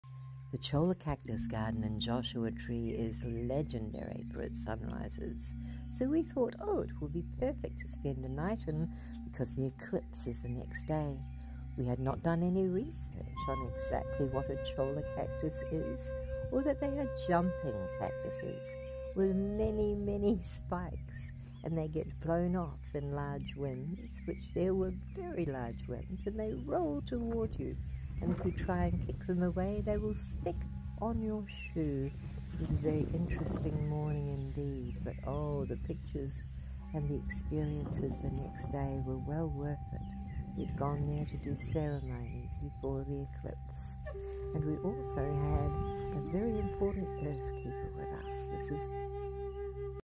sunrise at Cholla Cacti Gardens Joshua tree
Gustavo de la Torre and I spent the night amonst these intense chollas because we heard it was a fantastic sunrise spot
As evidenced by the glorious 1st photos Gustavo took
Next time we'll choose a less enthusiastically spiky spot. They are known as jumping cacti for a reason